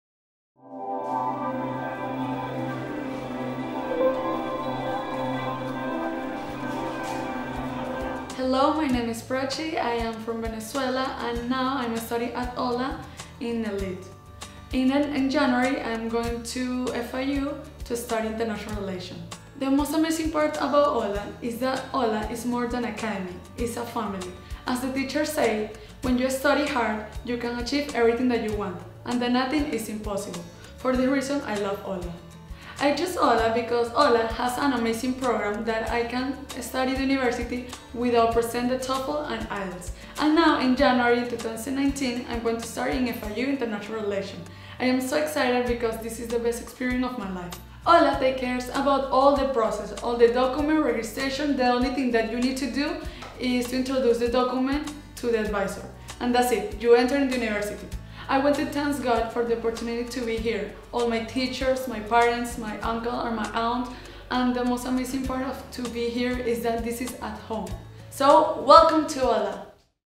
Prachi Lalwani Accepted to FIU
Congratulations to Prachi Lalwani for being accepted to FIU thanks to OHLA's University Placement Department.